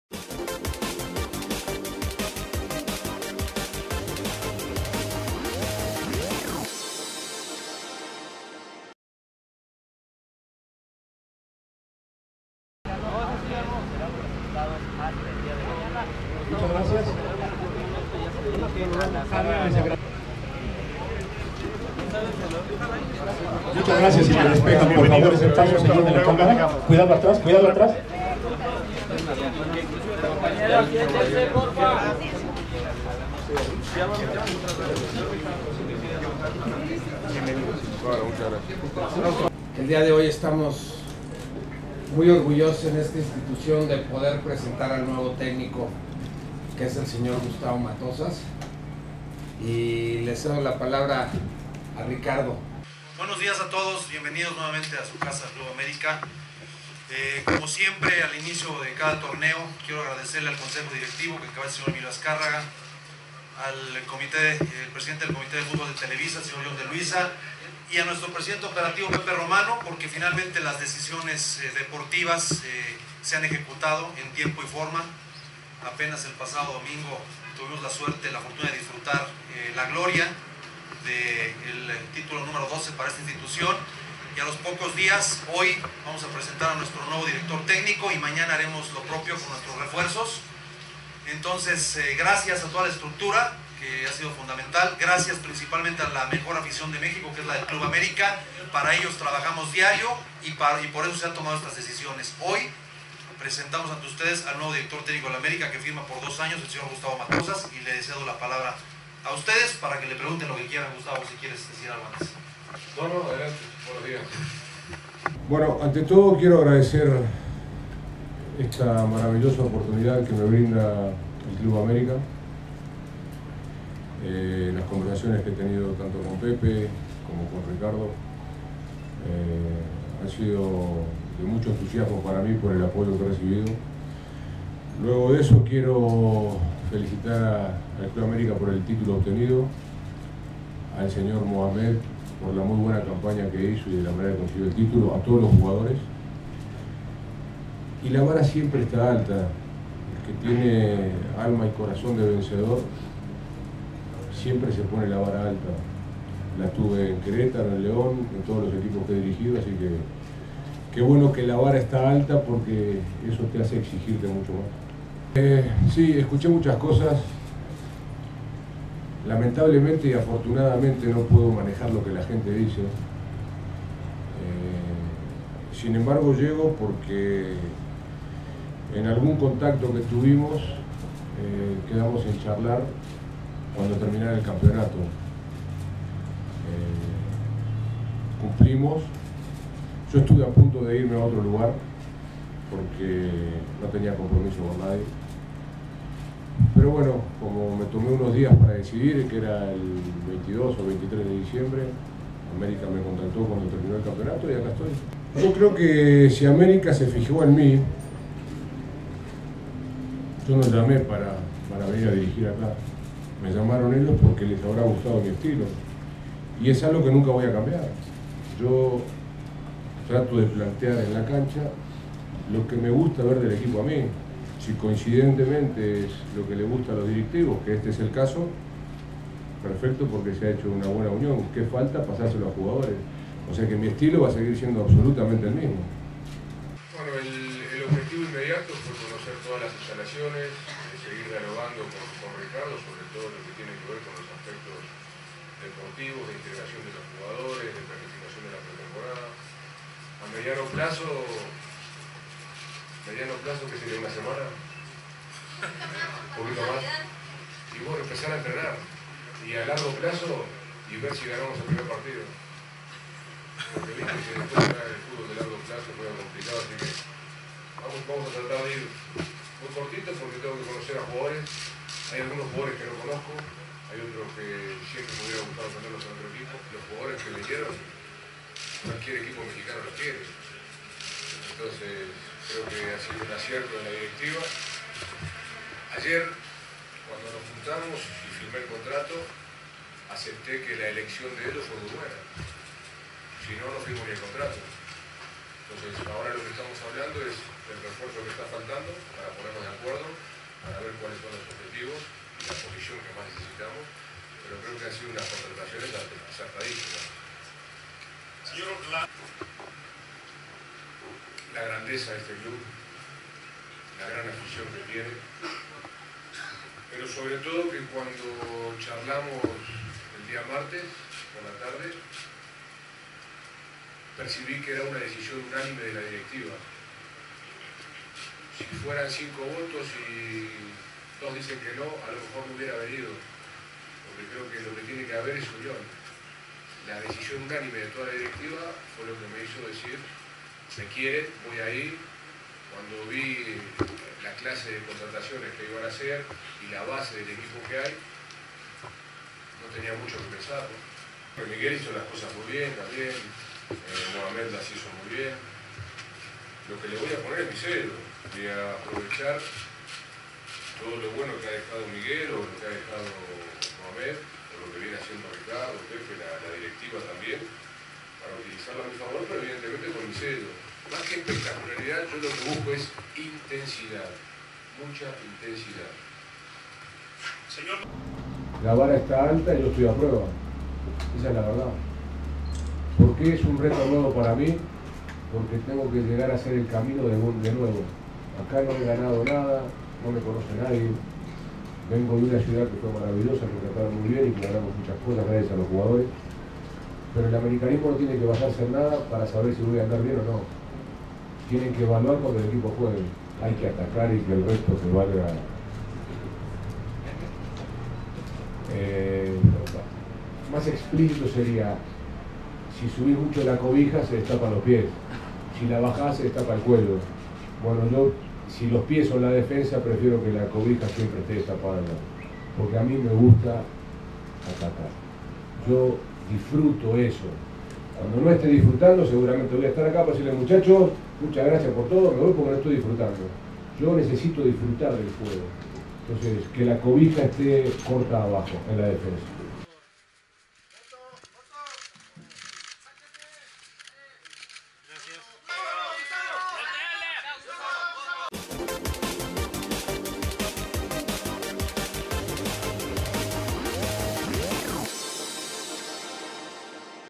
Conferencia de prensa presentación de Gustavo Matosas Club América
Gustavo Cristian Matosas Paidón es el nuevo Director Técnico del Club de Futbol América, el estratega uruguayo llegó oficialmente a nuestro club para dirigir al equipo campeón del futbol mexicano a partir del Clausura 2015.